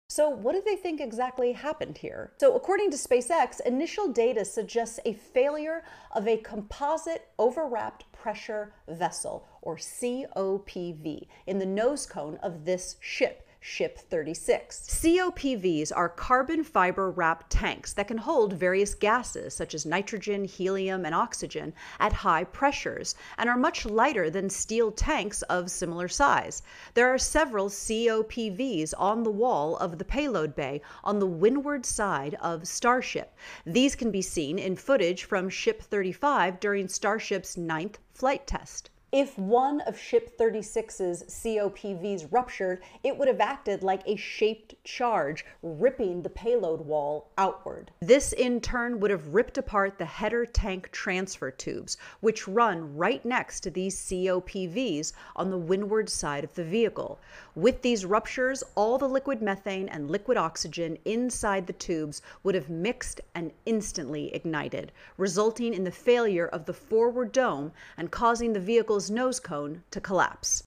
So, why did Starship go boom?
Part of a longer video about what's going on at SpaceX right now.

Think Geek Steampunk-style watch
Blue Yeti Microphone
Ring Light